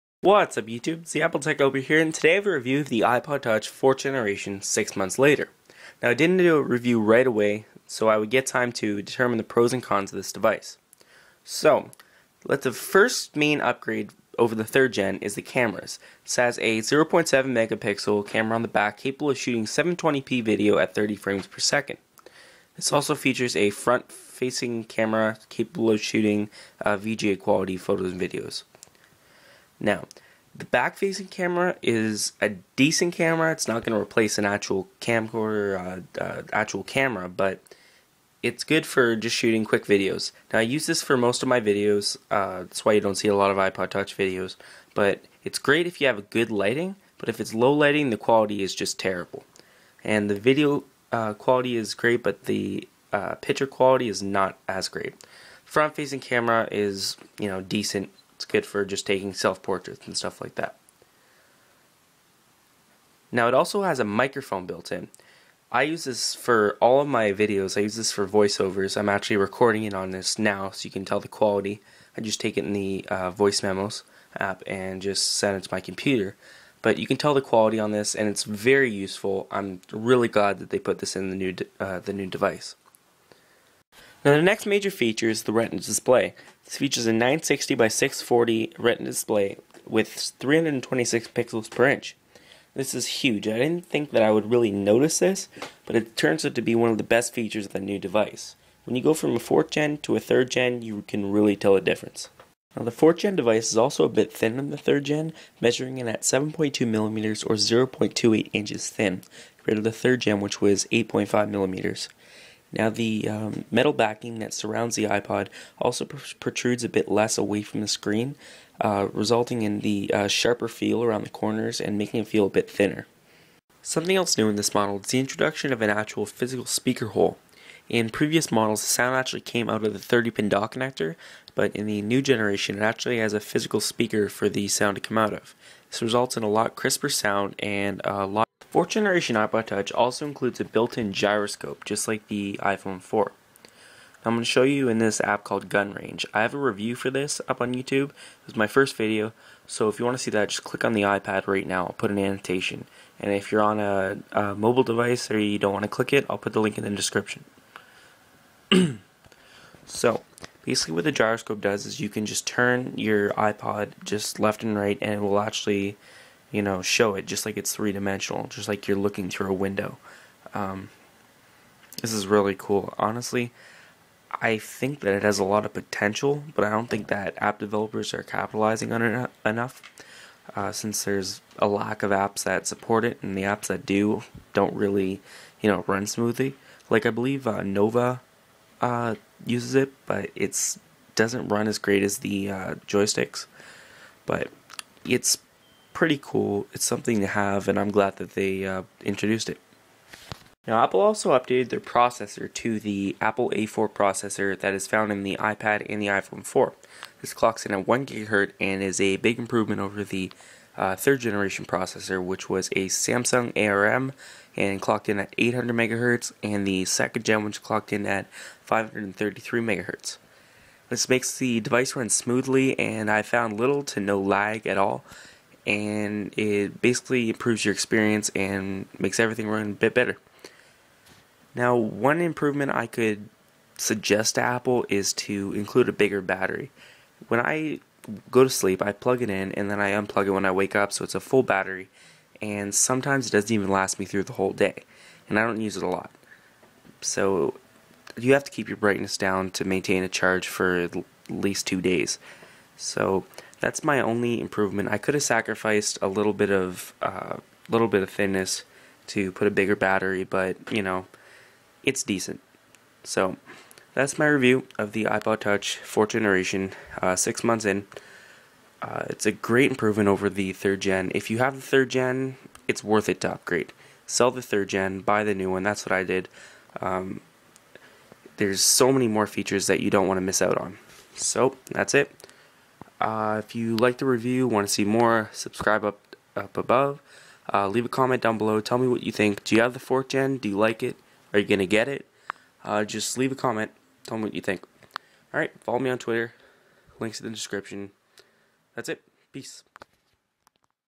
iPod Touch 4th Generation Review : 6 Months Later
This is my review of the iPod Touch 4th Gen. This is the by far the best portable music player on the market today and is by far worth the money.

Camera: The iPod Touch 4G features a 0.7 mp camera capable of shooting 720p video at 30 frames per second. It is a good camera in great lighting but aweful in low lighting conditions.

Retina Display: The iPod Touch 4G has adopted the 960 by 640, 326 pixels per inch retina display found in the iPhone 4. It looks amazing and is really something that you have to see in person to appreciate how great this looks.

Thinner Profile: The iPod Touch 4G is just 7.2mm (0.28 inches) thin and features a flat bottom for a wobble-free lay on the table design.

Gyroscope: The iPT 4G features a 3-axis gyroscope for improved gaming. This is a cool feature that I feel deserves more attention.

A4 Processor: The iPT 4G  is powered by a 1GHz A4 processor and is the same processor found in the iPhone 4 and the iPad.